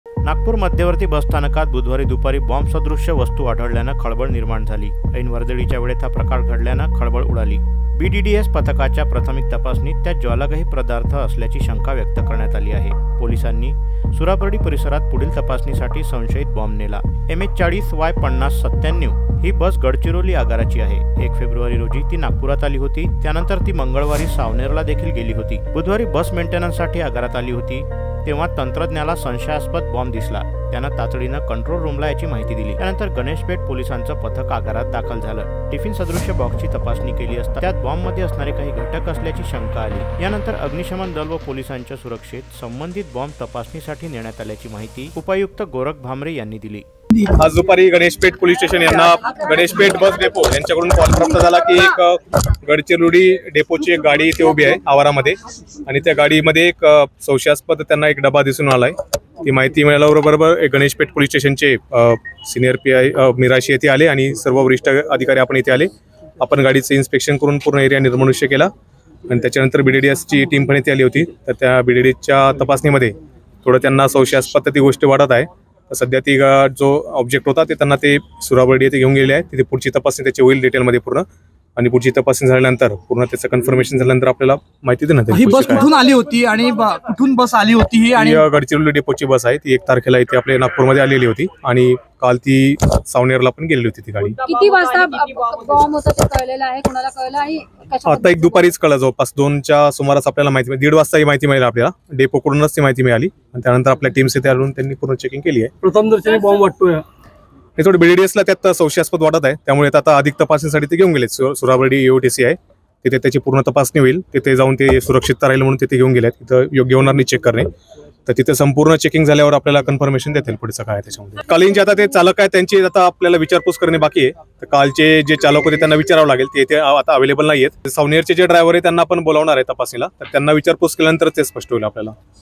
नागपूर बस स्थानकात आढळला संशयित बॉम्ब, शहरात खळबळ#nagpur
नागपूर मध्यवर्ती बस स्थानकात बुधवारी दुपारी बॉम्बसदृश्य बॉक्स आढळल्याने खळबळ उडाली. ऐन वर्दळीच्या वेळेत हा प्रकार घडल्याने खळबळ उडाली. बीडीडीएस पथकाच्या प्राथमिक तपासणीत त्यात ज्वालाग्राही पदार्थ असल्याची शंका व्यक्त करण्यात आली आहे. पोलिसांनी सुराबर्डी परिसरात पुढील तपासणीसाठी संशयित बॉक्स नेला आहे.
एमएच ४० वाय ५०९७ ही बस गडचिरोली आगाराची आहे. १ फेब्रुवारी रोजी ती नागपुरात आली होती. त्यानंतर ती मंगळवारी सावनेरलादेखील गेली होती. बुधवारी बस मेंटेनन्ससाठी आगारात आली. तेव्हा तंत्रज्ञाला संशयास्पद बॉक्स दिसला. त्याने तातडीने कंट्रोल रूमला याची माहिती दिली. त्यानंतर गणेशपेठ पोलिसांचे पथक आगारात दाखल झाले. टिफिन सदृश्य बॉक्सची तपासणी केली असता त्यात बॉम्बमध्ये असणारे काही घटक असल्याची शंका आली. त्यानंतर अग्निशमन दल व पोलिसांच्या सुरक्षेत संबंधित बॉक्स तपासणीसाठी नेण्यात आल्याची माहिती उपायुक्त गोरख भामरे यांनी दिली.